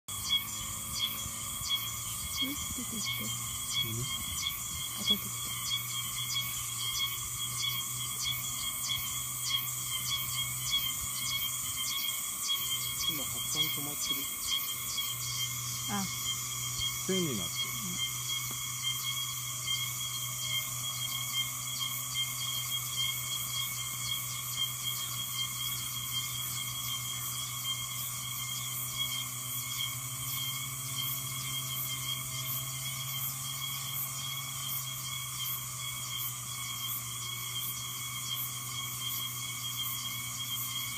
リュウキュウウラナミジャノメ / Ypthima riukiuana 沖縄乙羽岳 2015/09/29 butterfly of Japan, Okinawa
A Wild Butterfly Chase　蝶をめぐる冒険
butterfly of Japan　日本の蝶　沖縄の蝶